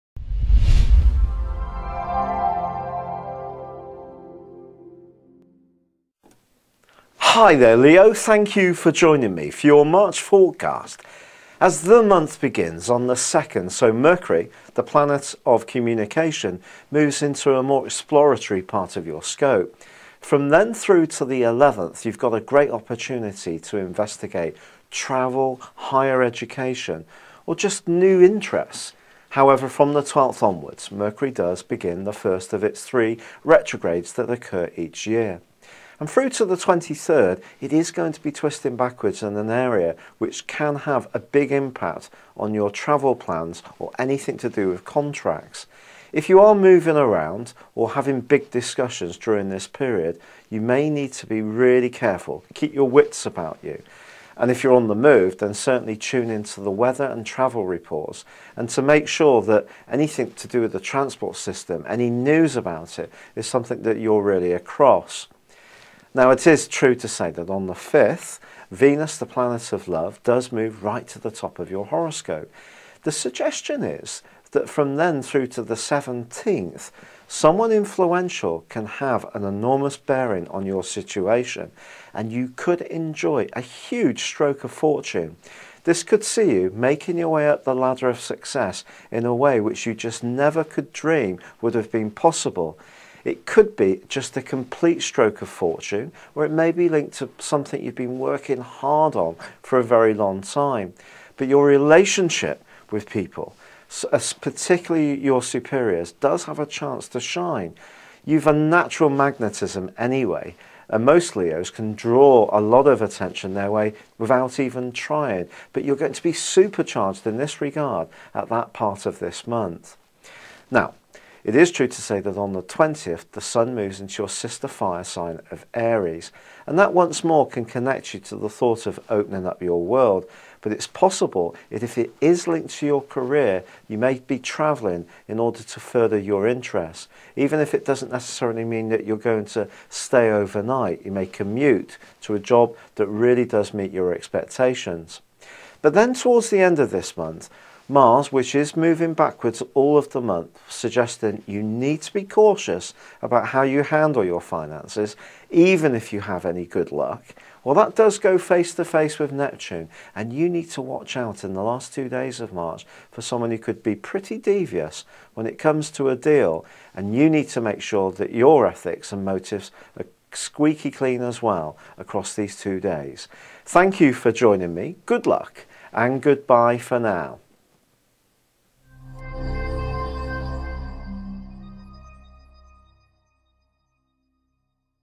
Leo March 2012 Horoscope Video Forecast
Leo Monthly Horoscope February 2012. Also watch your Leo Weekly Horoscope and Year 2012 Horoscope Video with me, Astrologer Patrick Arundell.